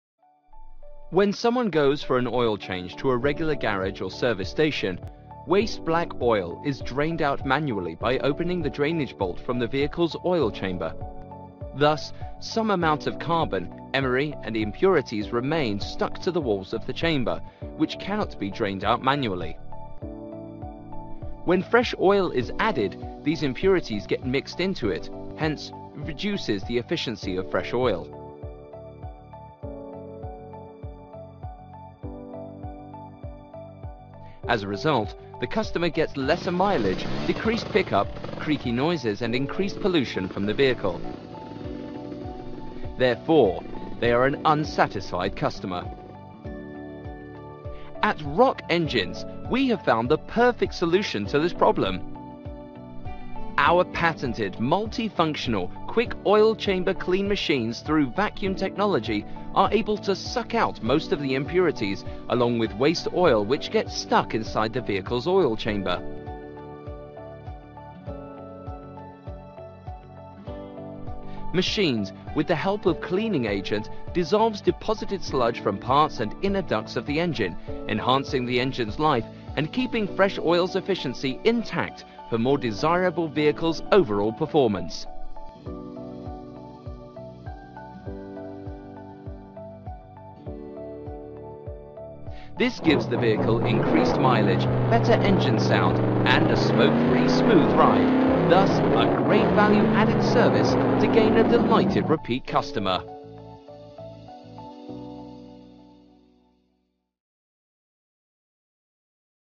Quick Oil Change Machine Animation (English) | Rock Engines™
Rock Engines™ Quick Oil Chamber Clean Machines are created for a range of applications: from 2-Wheelers & 4-Wheelers to HMV and industrial DG Sets and boats. They provide you an affordable and easy-to-use solution which are meant to delight customers and develop businesses.

For more videos on bike/car/HMV oil change and engine flushing, visit our YouTube channel